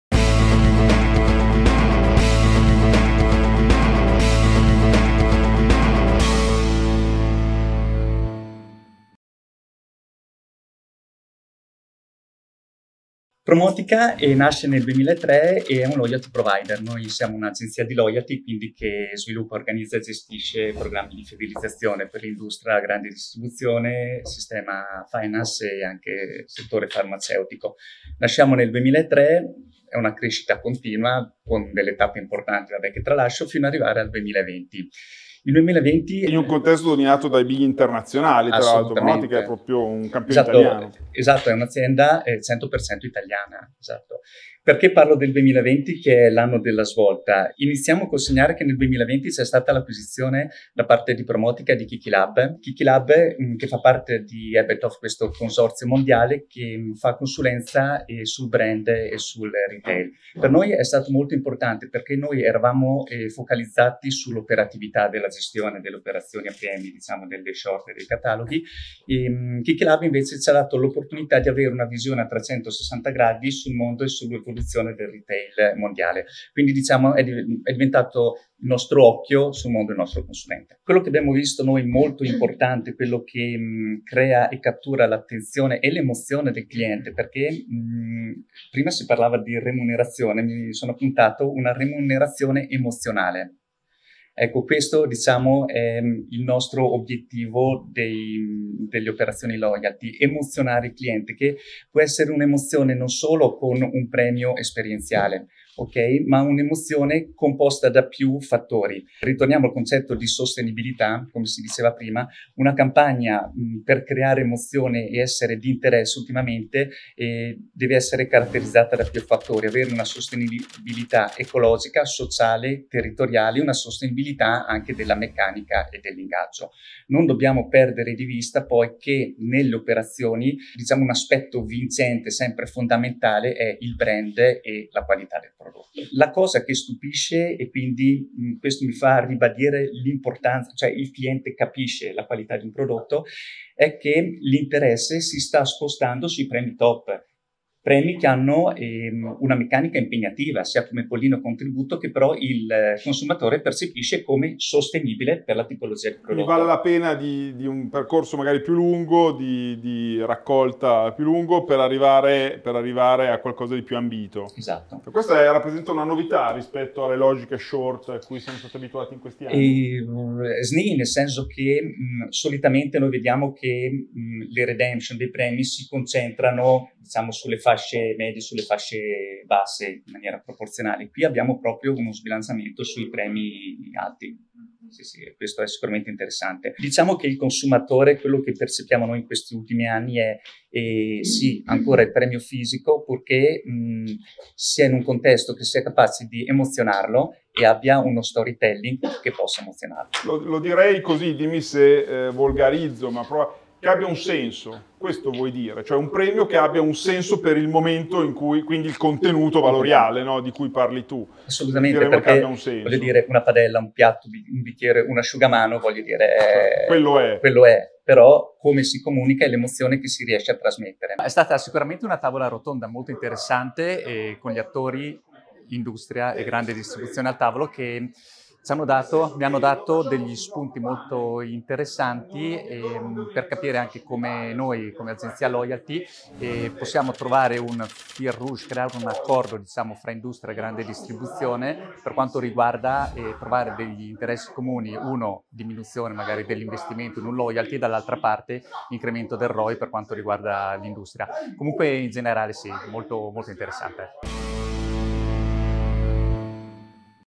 Alessandro Tiefenthaler, Promotica: “Obiettivo delle operazioni di loyalty è emozionare il cliente”
Forum:
Loyalty Management Circle
Milano, 20 aprile 2022